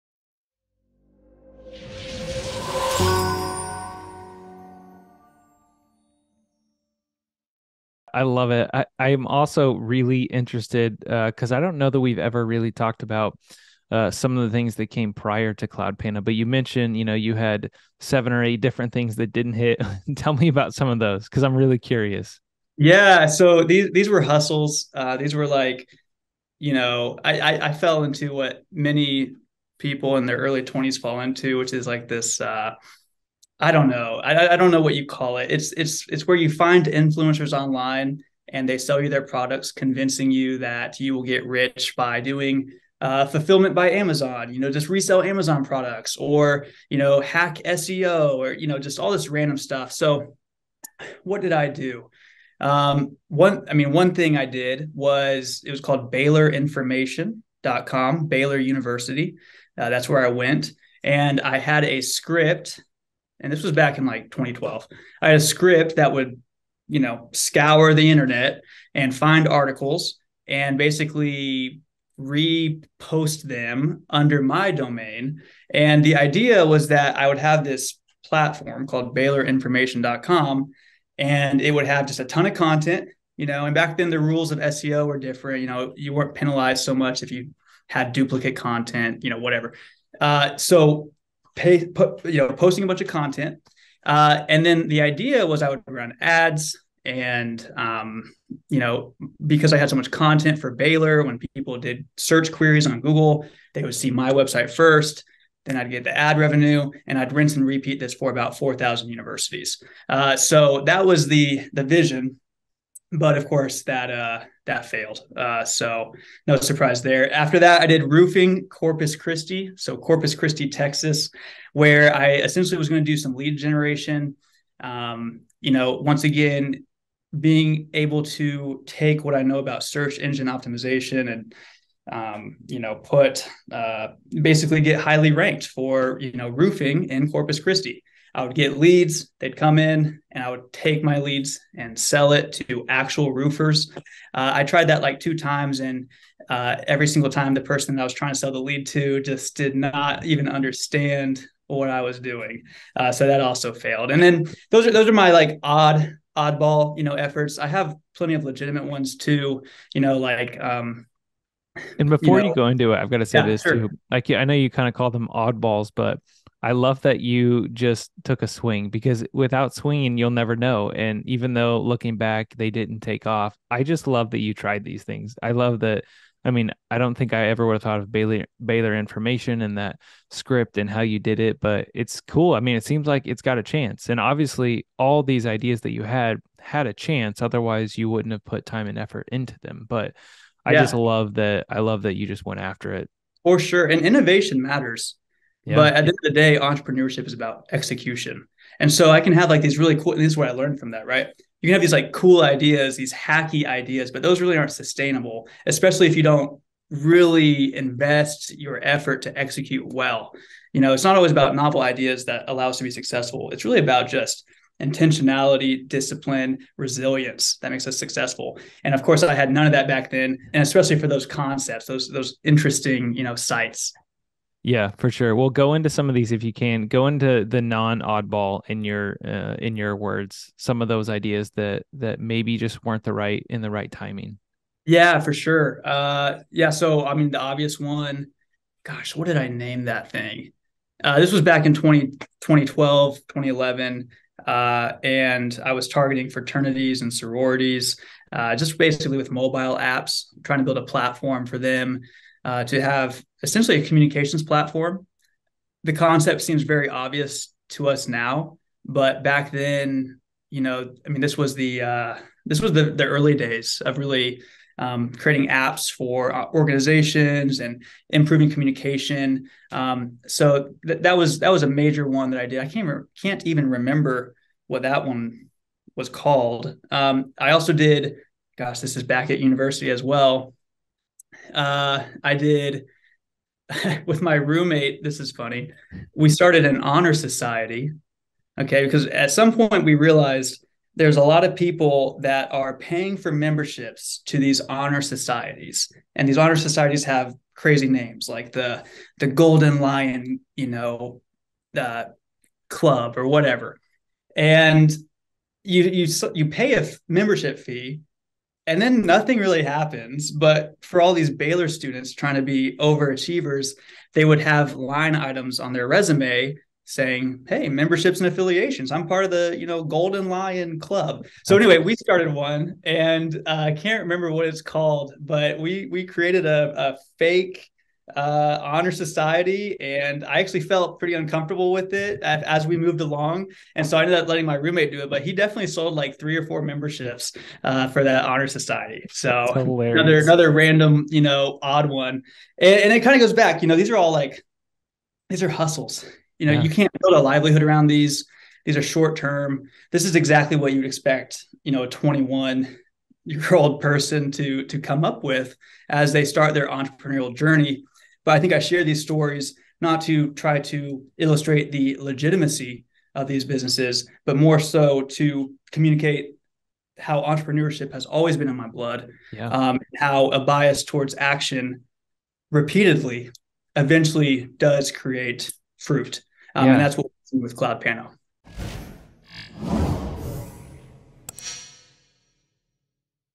EP 85: Clayton Rothschild - Side Hustles
Clayton shares about some of his early side hustles, including his idea for a website called Baylor Information.

Clayton Rothschild is an entrepreneur from Springfield, MO. He is very interested in the creative destruction and disruptive effect technology has on everyday life. His  favorite topics are those that are perhaps undergoing the most disruption: accessibility, economics, consumer behavior, and regulation.

He is passionate about product development. He is also the co-founder and co-owner of CloudPano, the standard in 360º virtual tour software. CloudPano has been seen on Redfin and Zillow, and has clients across the globe.